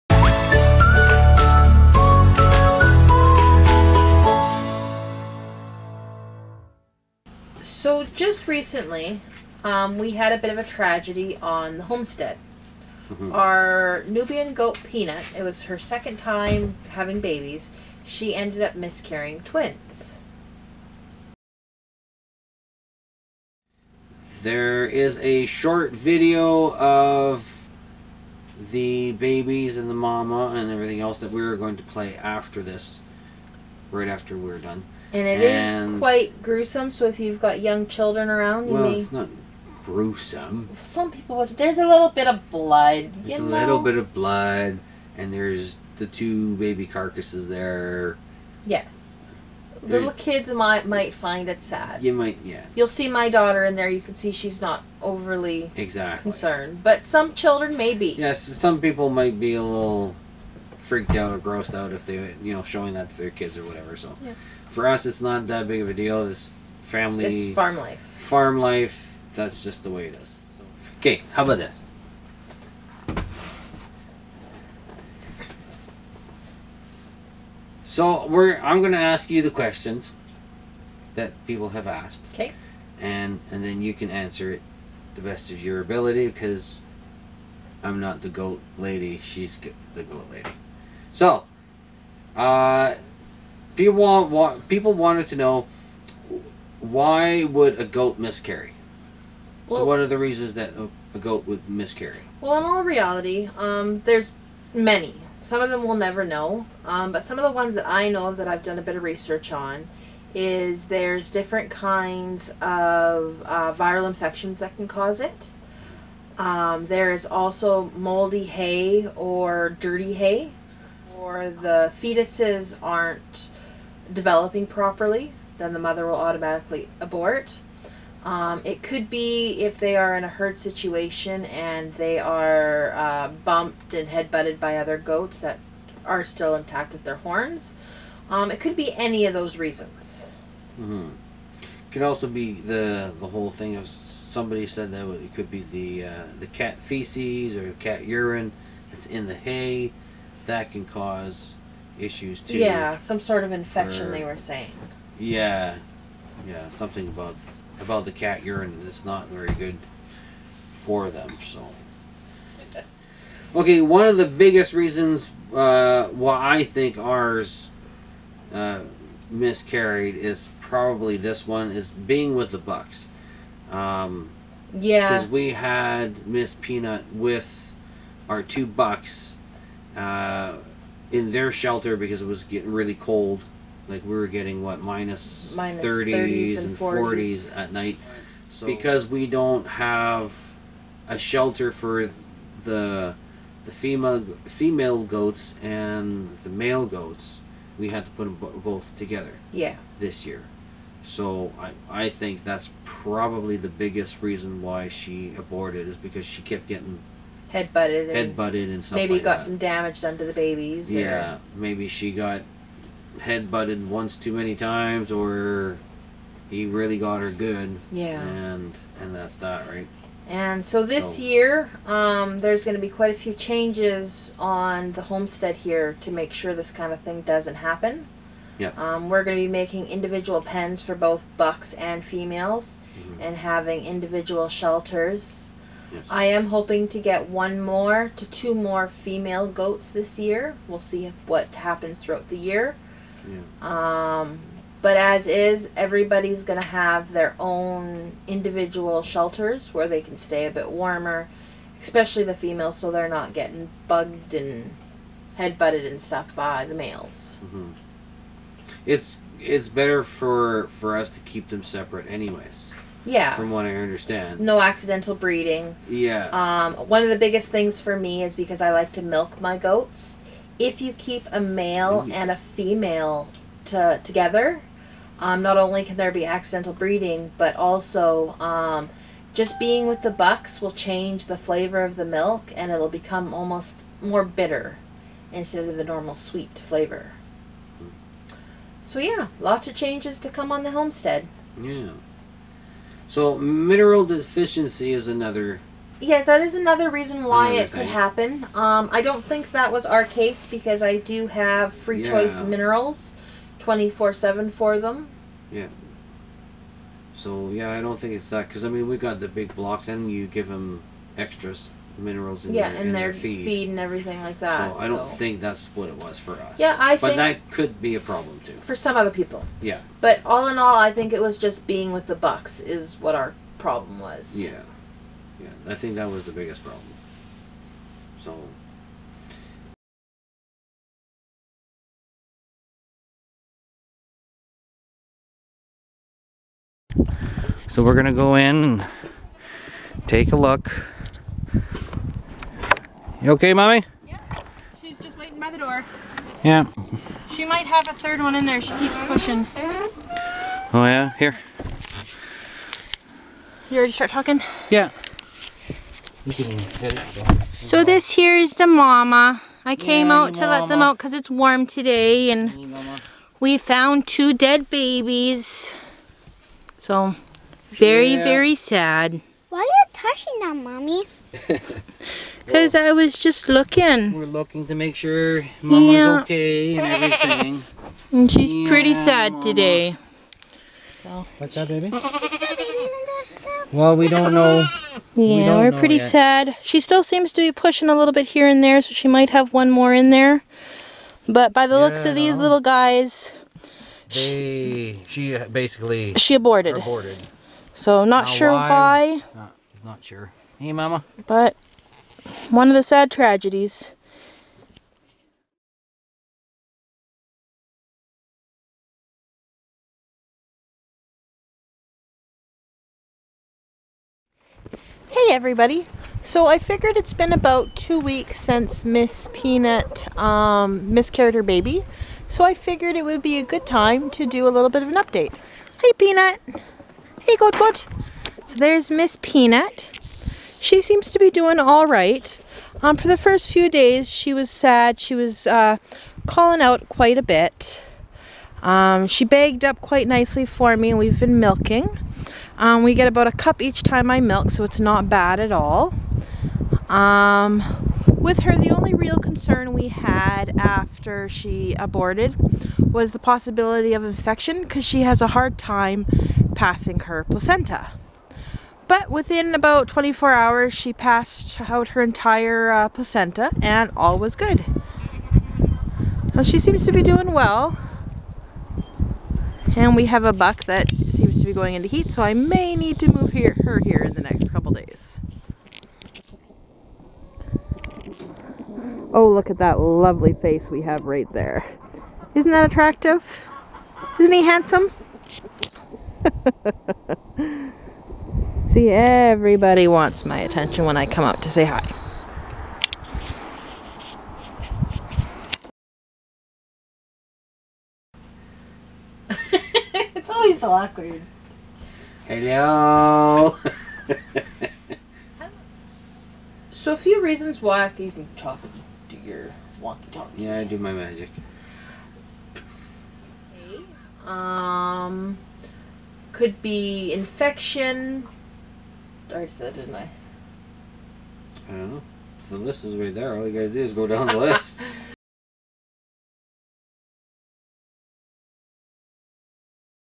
So Our goat had a Miscarriage
On the off grid homestead we had a bit of a sad day and our goat Peanut miscarried her  Twins:( So we discuss the possible causes of her miscarriage